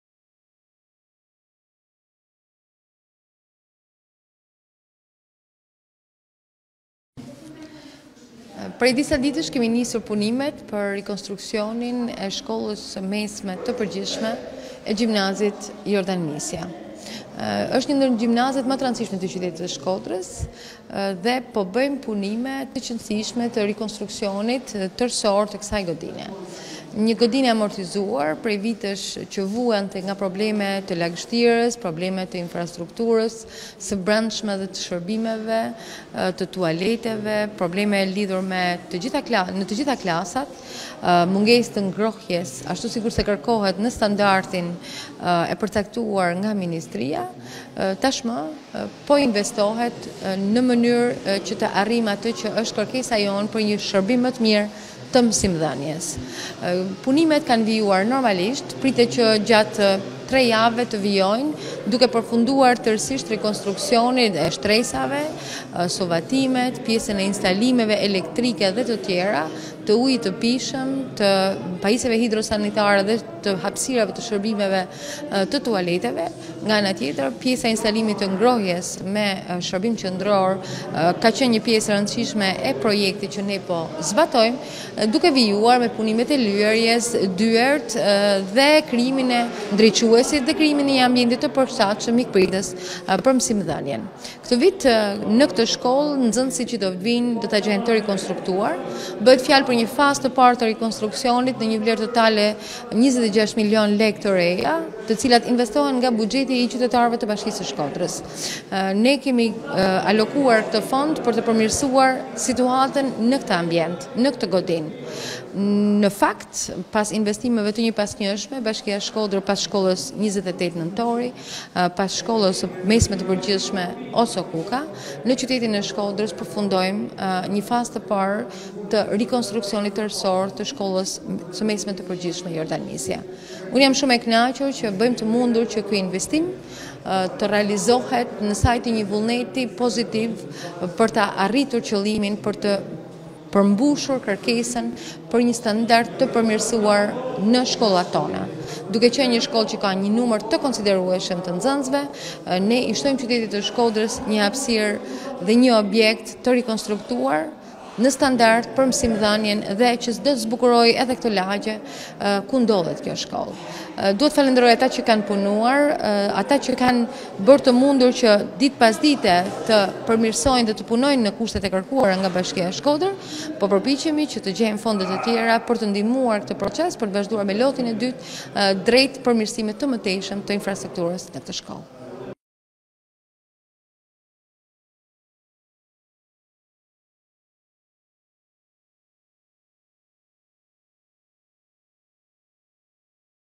ShkodraWeb Media IBashkia Shkodër jep 26 milion lekë për rikonstruksionin e gjimnazit "Jordan Misja"
Me një fond 26 milion lekë nga të ardhurat e Bashkisë së Shkodrës, ka nisur dhe po vijon rikonstruksioni i gjimnazit “Jordan Misja” në qytetin e Shkodrës.
Ndërhyrja ka qenë e domosdoshme, pasi shkalla e amortizimit të ambjenteve të brende dhe të jashtme të ndërtesës ka qenë mjaft e lartë.
Projekti i Bashkisë Shkodër parashikon riparim apo zëvendësim të shtresave të suvasë, zëvendësim i të gjitha pllakave të dyshemeve, zëvendësimin e rrjetit të vjetër elektrik dhe vendosjen e ndriçuesve LED, instalimin e rrjetit të kaldajës dhe i sistemit të hidrantëve për mbrojtjen kundër zjarrit, rikonstruksioni i plotë i terracës përfshi hidroizolim e saj.
Me rifillimin e procesit mësimor, mësues, nxënës e prindër të gjimnazit “Jordan Misja” në qytetin e Shkodrës, do të kenë në dispozicion një ambjent krejtësisht të rikonstruktuar.
Rikosntruksioni i kësaj shkolle, vjen pas rikonstruksionit të dy gjimnazeve të tjera të Shkodrës, “28 Nëntori” dher “Oso Kuka”.